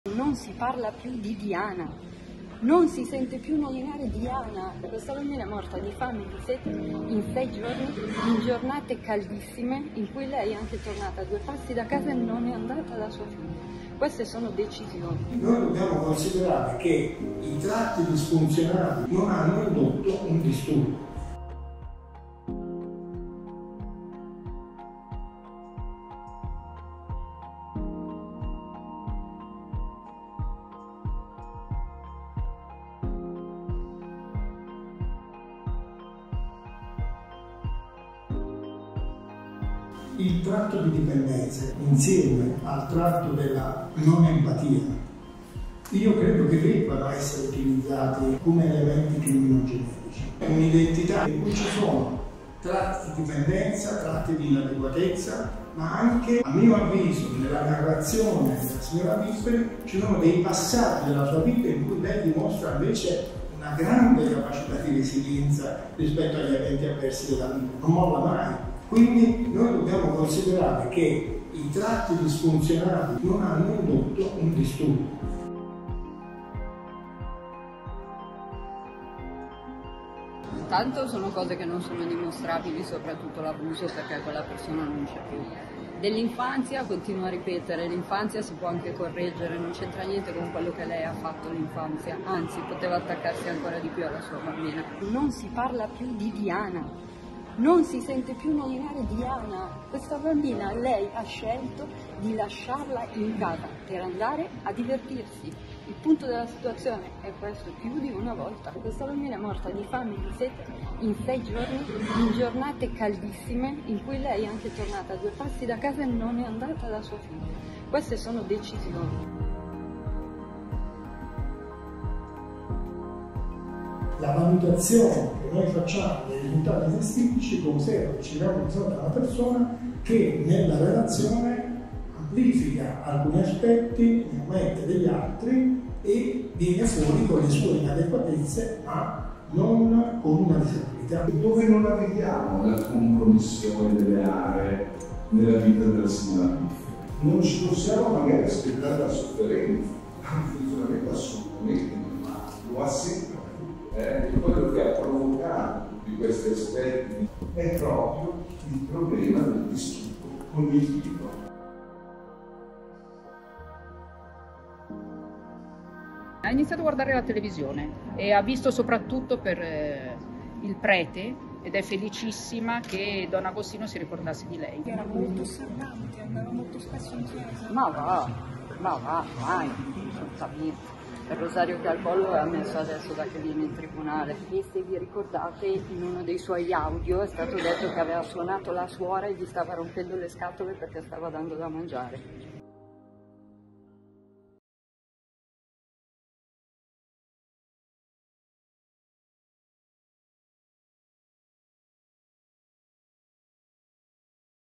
Processo Pifferi, lo psichiatra della perizia: "Tratti di personalità, nessun disturbo cognitivo"
Nell’udienza del processo su Alessia Pifferi di oggi venerdì 15 marzo si sta tenendo l’interrogatorio al medico Elvezio Pirfo che, tramite una perizia psichiatrica superpartes, ha ritenuto l’imputata capace di intendere e di volere quando ha abbandonato la figlia di 18 mesi per giorni causandone la morte per stenti.